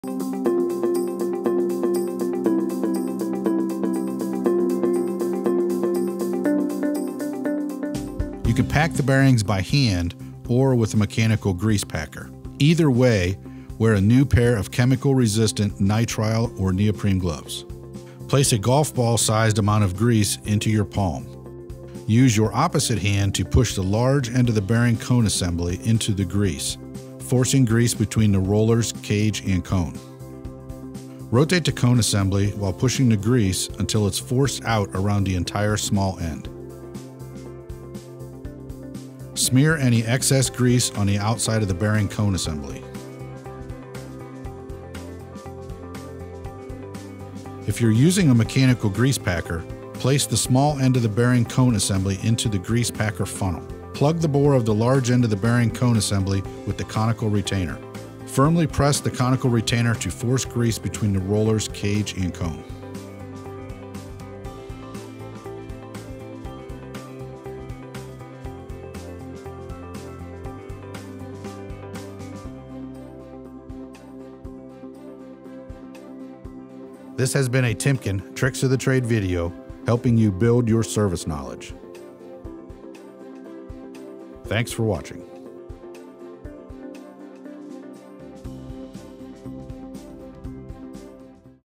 Timken Tricks of the Trade – Lubricating Wheel End Bearings
Proper lubrication greatly increases the life of bearings, especially in adverse conditions. This video will show you how to pack wheel bearings with grease – by hand and with a mechanical grease packer.